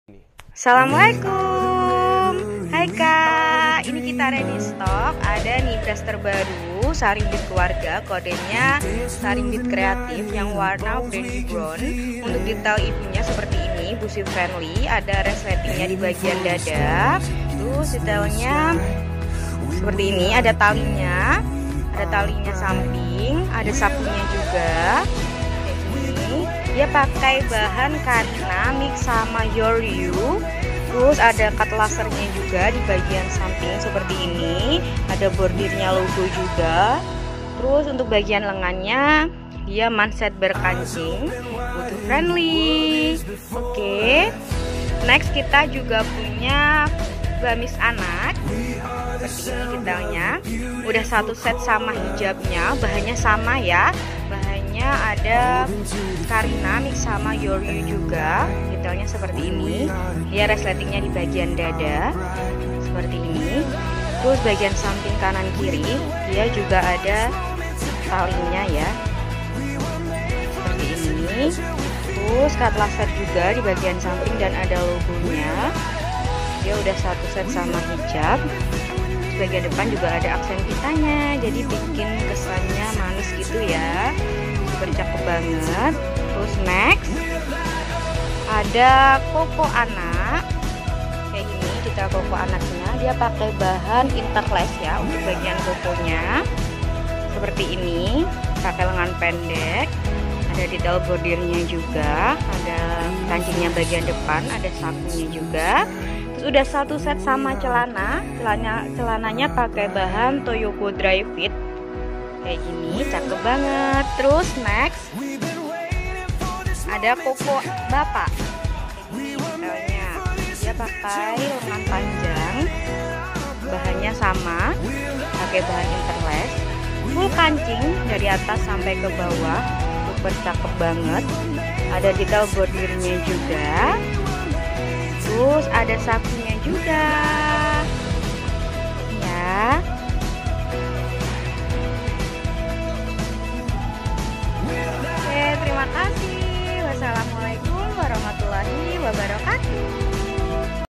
SARIMBIT NIBRAS CREATIVE
CREATIVE GAMIS

Harga Ecer Rp 258,000

*Material Youryou Mix Carina*
- Bahan kain Youryou karakternya hampir mirip kain satin dan sutera (lebih tebal dari satin, mengkilat, jatuh di badan, ringan dan mewah) ini sangat nyaman digunakan dengan tekstur seperti kulit jagung.
- Karakter bahan Carina ini lembut, jatuh, adem Handfeel yang lembut membuat nyaman saat beraktivitas.

Detail Produk :
- Busui Friendly
- Saku Sisi
- Lengan Manset dan Kancing
- Variasi Bordir dan Lasercut
- Variasi Tali

*Kesesuaian foto dan asli 90 - 100% dipengaruhi faktor cahaya pemotretan, editing dan resolusi cahaya hp masing-masing
*Kesesuaian standard sizepack dengan asli 1-2cm